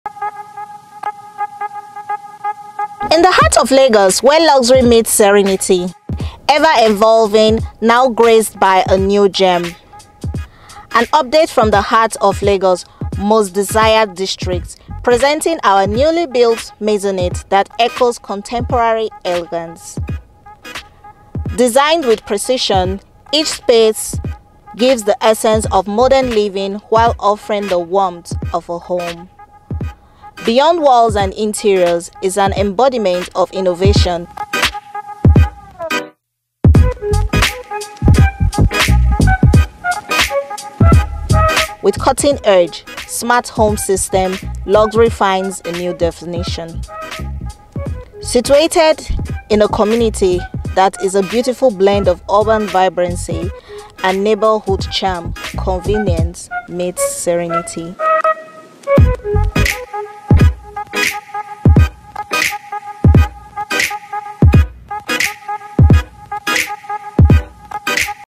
4BEDROOM TERRACES FOR SALE AT OCEAN BAY ESTATE, ORCHID, LEKKI, LAGOS.
HILLSTAR COURT 2

Luxurious 3 bed maisonette and 4 bed maisonettes 80M and 85M respectively and 4 bedroom penthouse apartments 110M

Location: Ocean Bay Estate, Orchid, Lekki.

Unique Features:

Dining Area
Ample parking space
Maid room/ BQ
In-built ceiling speakers
CCTV cameras
Private compound space
24/7 power
24/7 security
Central water system
Central sewage system
Recreational Facilities
Fully serviced estate
Gated and secured area

*Neighborhood and Landmarks*:

Chevron Tollgate
Orchid hotel
Prime mall
Mega chicken
Lekki conservatory center

Title : Governors Consent

Price -80M, 85M and 110M

To connect with MW HOMES AND REALTY